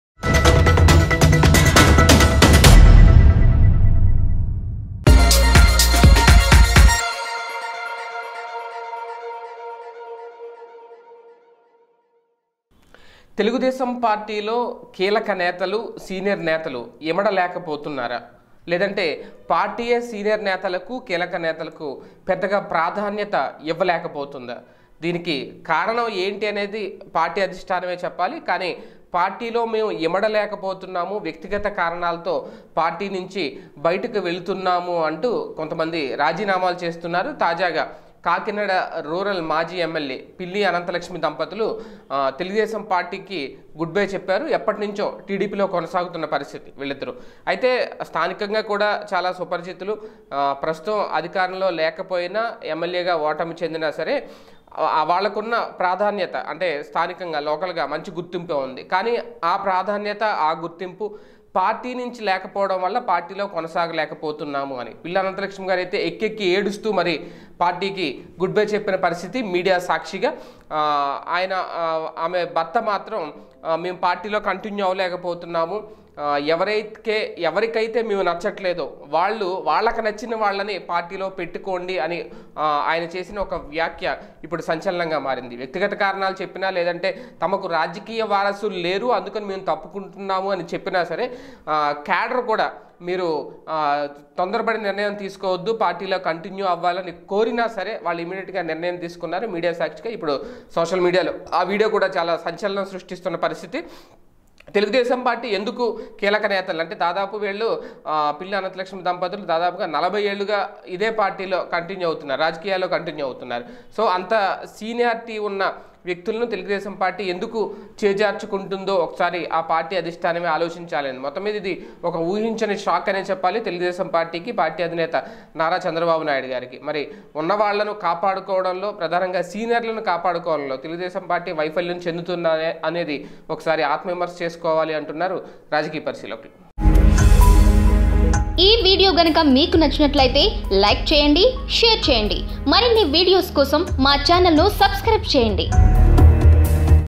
Former TDP MLA Resigned to TDP | Chandrababu | చంద్రబాబుకు కోలుకోలేని షాక్ | Anantha Lakshmi Pilli
చంద్రబాబుకు కోలుకోలేని షాక్.. టీడీపీలో ఇమడలేం, ఎక్కెక్కి ఏడుస్తూ రాజీనామా